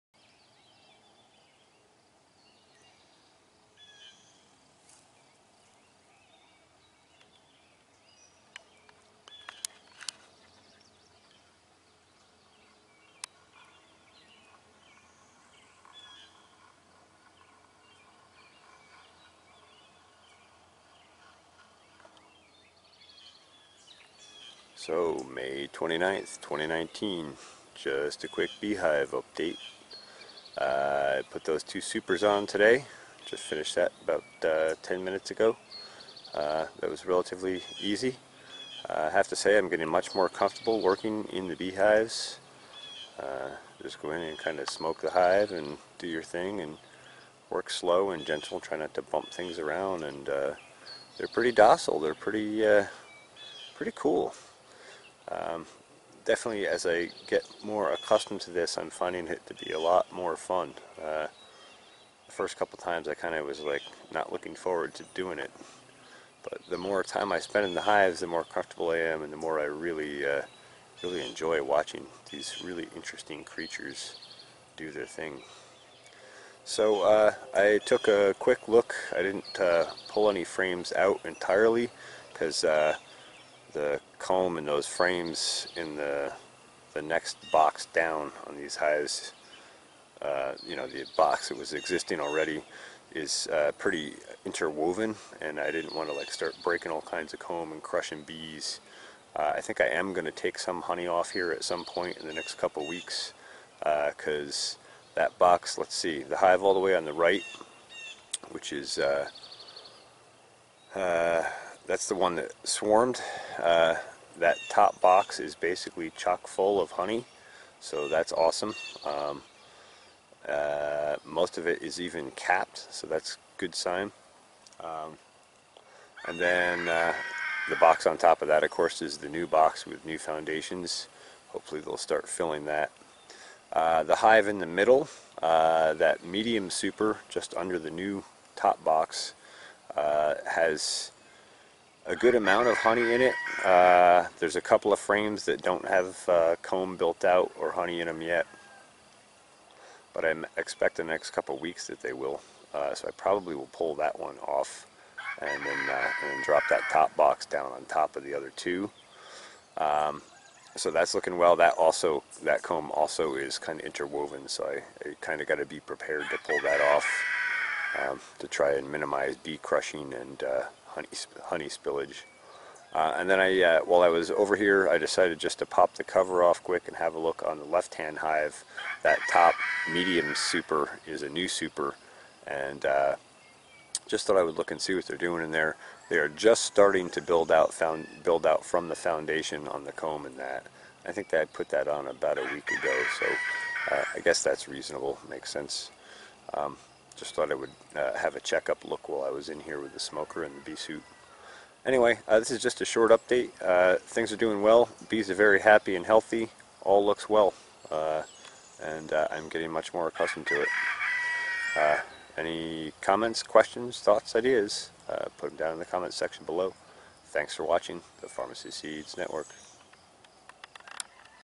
Honey Bee Hives Update New Deep Supers On *Status Report*
Just a quick update on the hives. I added 2 new supers to the last 2 hives. I also talk a little about current honey resources.